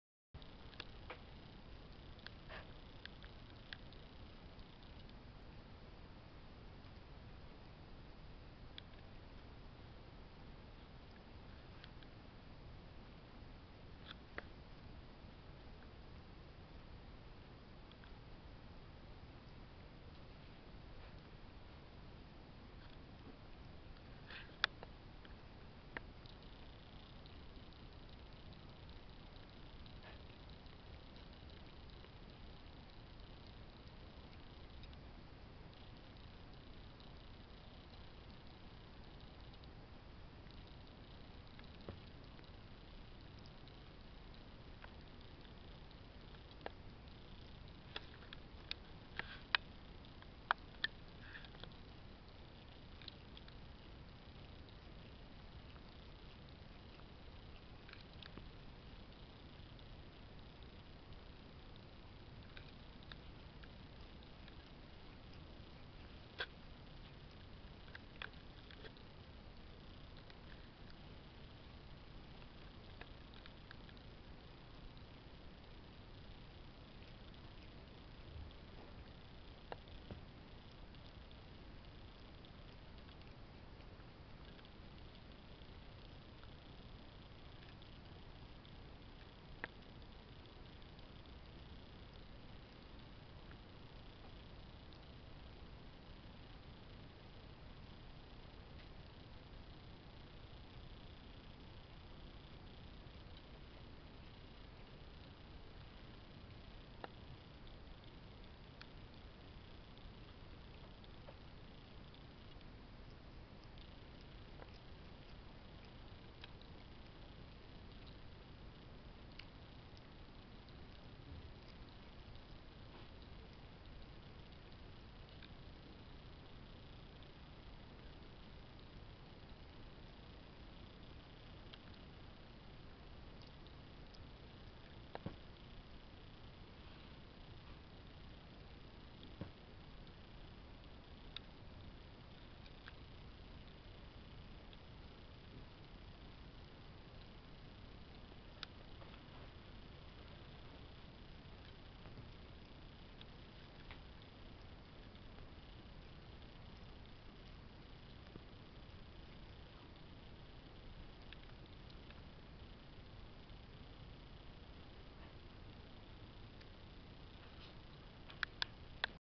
Bathtime
My cat Milliano bathes herself for the 10th time today. :)